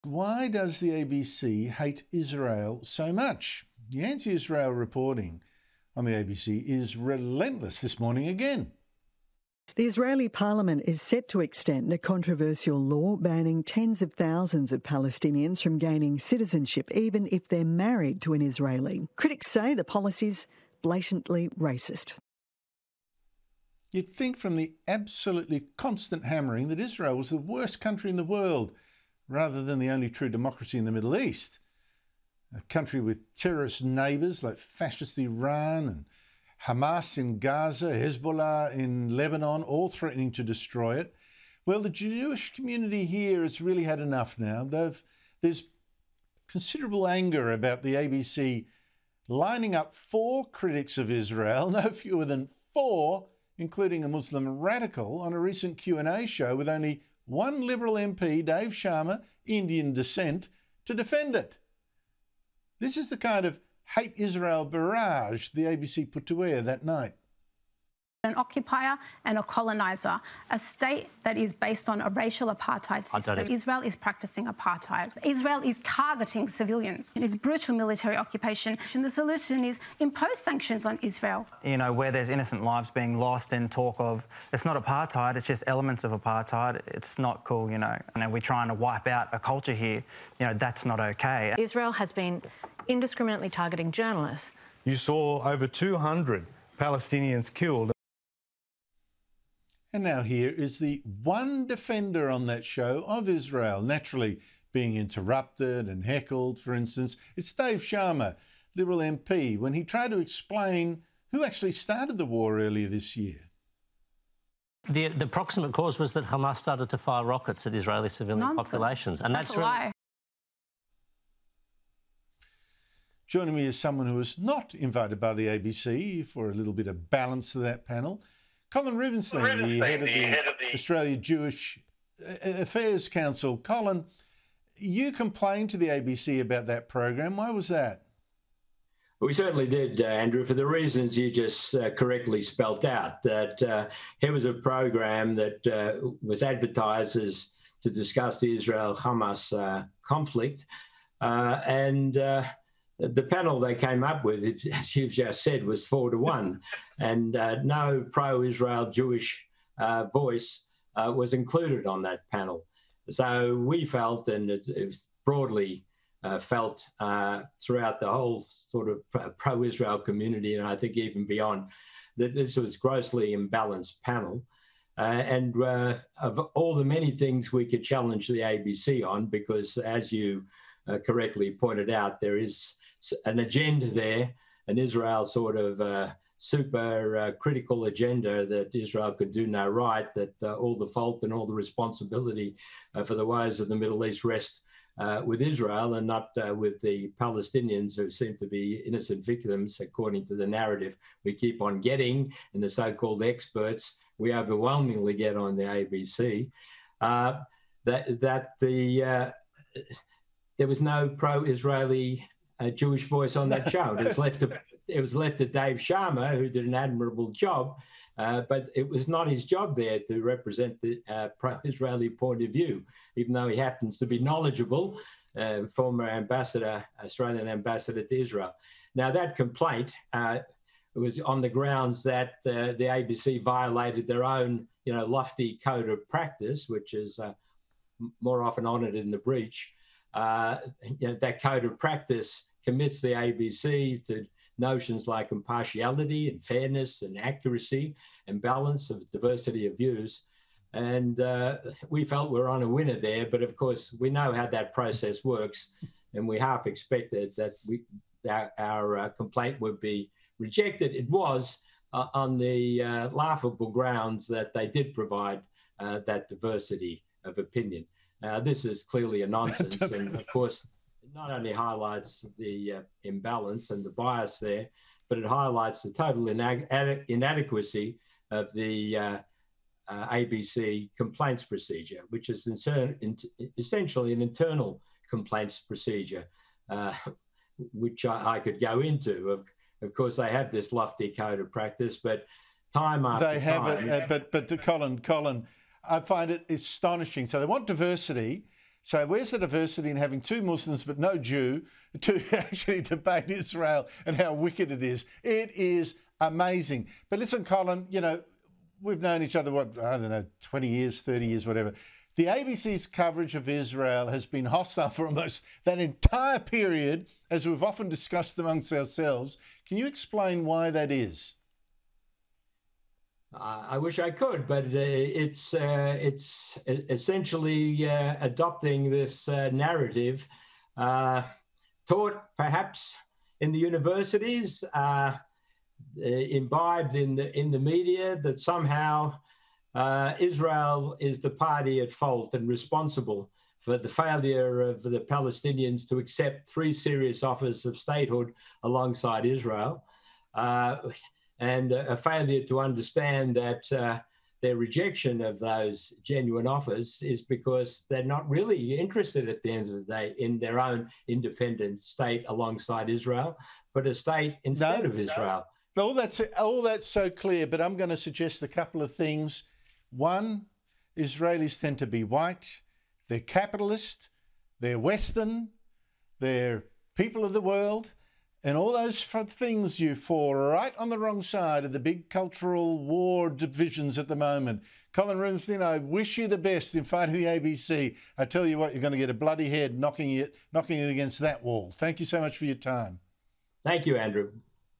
Jewish community have 'really had enough' of ABC's 'anti-Israel' stance
Sky News host Andrew Bolt says the Jewish community in Australia have "considerable anger" about the ABC lining up "four critics of Israel" on a recent QandA show – with only one individual to defend it.

"Why does the ABC hate Israel so much," Mr Bolt said.

"You'd think from the absolutely constant hammering Israel was the worst country in the world, rather than the only true democracy in the Middle East – with terrorist neighbours like fascist Iran, Hamas in Gaza, Hezbollah in Lebanon all threatening to destroy it.

"The Jewish community here has really had enough now, there's considerable anger about the ABC lining up four critics of Israel, no fewer than four, including a Muslim radical on a recent QandA show.

"And only one Liberal MP Dave Sharma, Indian descent – to defend it."

Mr Bolt spoke with the head of the Australia Israel & Jewish Affairs Council Dr Colin Rubenstein on the matter.